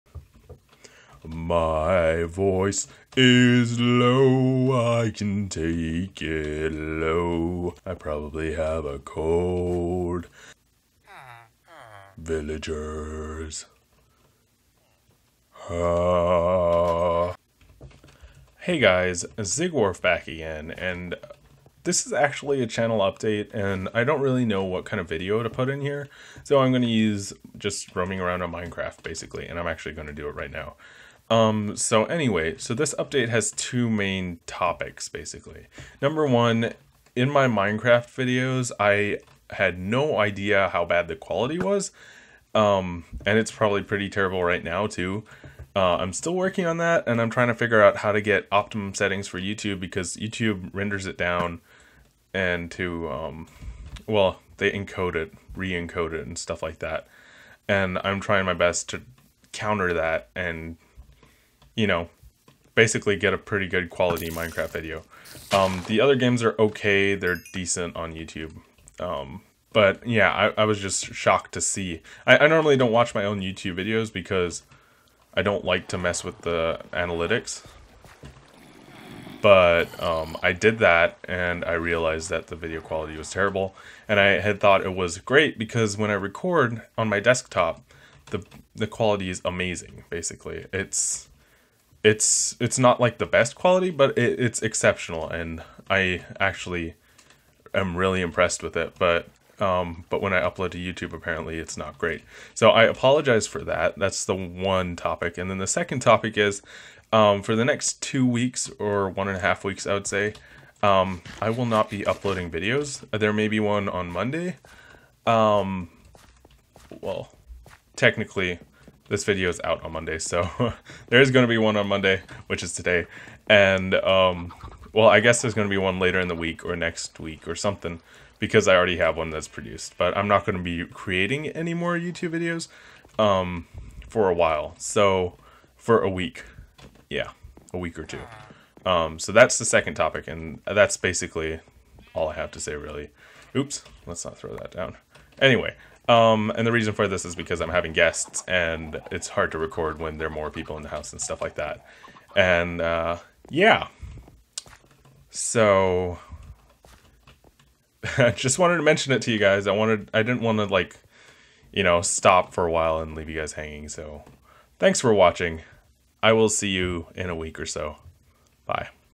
Channel Update #3 - Another break and Quality...
Channel Update! - In this one i apologize for the terrible video quality of my minercaft videos and announce another break.

Version: Snapshot 1.9.2